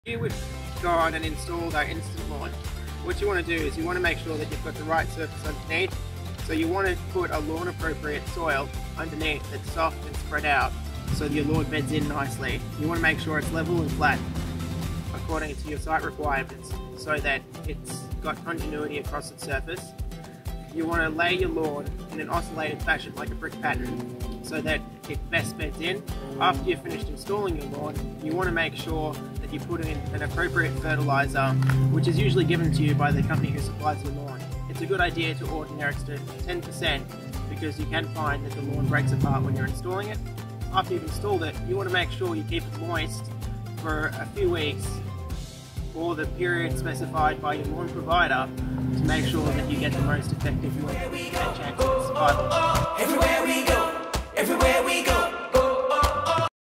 Installing an Instant Lawn
Make an impression on the first thing you see when coming home: Your nature strip.
This is one of the many places to install instant lawn.
Follow our steps for a quick, easy home improvement.